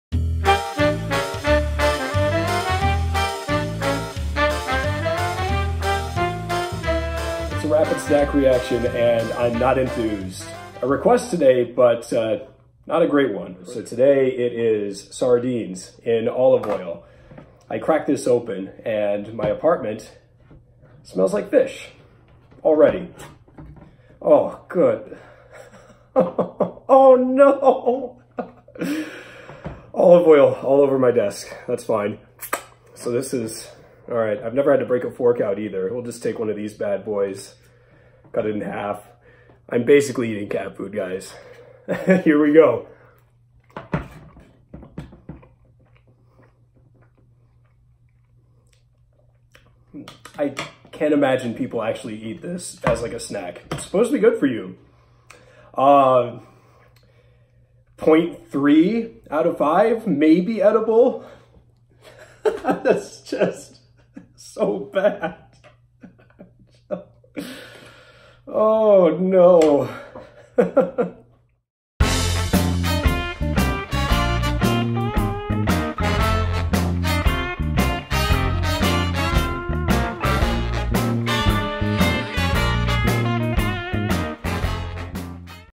Snack Review: Sardines in Olive Oil
A snack review of Sardines in Olive Oil (by request) from Trader Joe's. This is the lowest score I've given in this series so far. Sardines are supposed to be healthy and a great source of protein. but this just isn't worth it for me. Plus...the olive oil makes the texture even WORSE.

Have a suggestion of what I should try next?